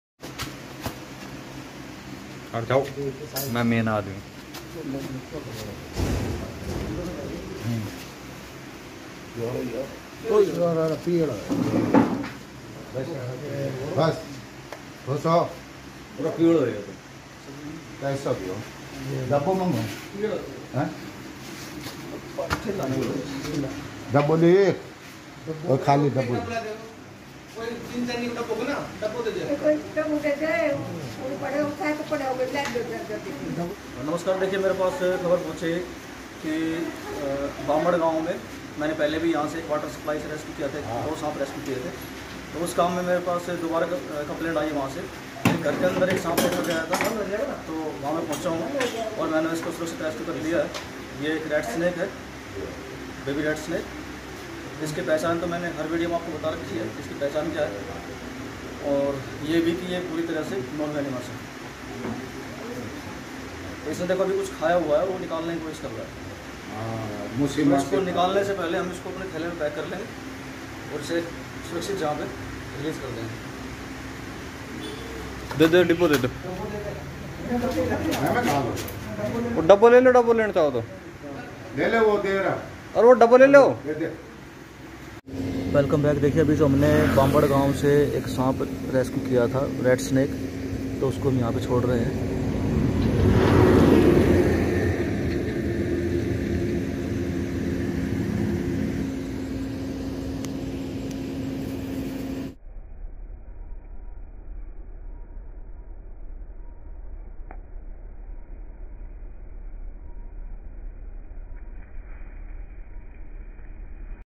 गाँव बांबड़ में एक घर में पशुओं के तूड़े में था ये साँप , This snake was in the garbage of animals
गाँव बांबड़ में एक घर में पशुओं के तूड़े में था ये साँप , In a house in the village of Bambad, this snake was in the garbage of animals.